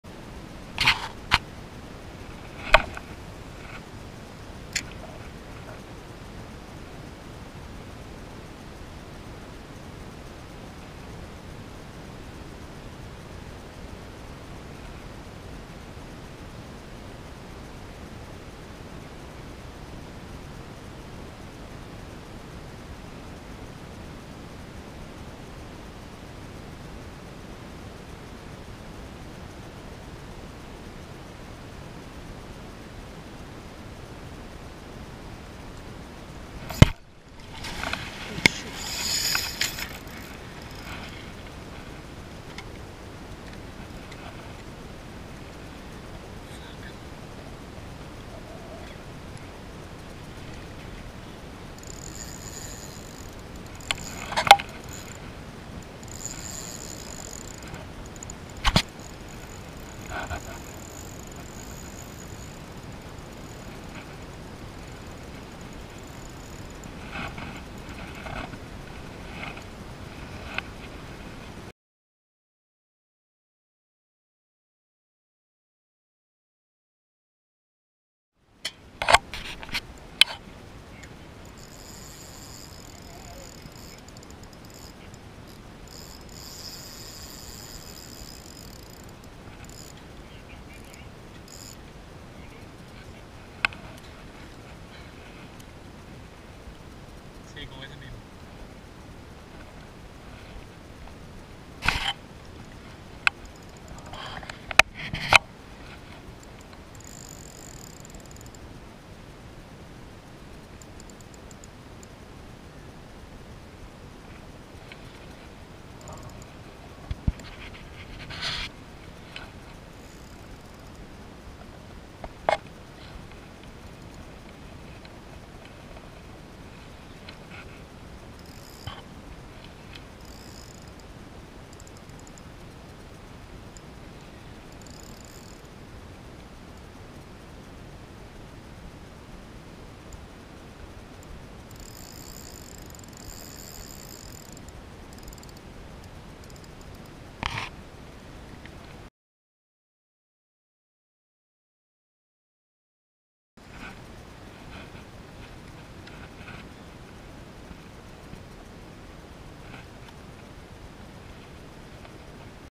Chokoloskee Tarpon
One of the most aggressive runs I've ever experienced from a kayak. This fish looked like a 60-70lbs easy, no match for my 15lbs outfit...  lost it at the end as it spit the hook.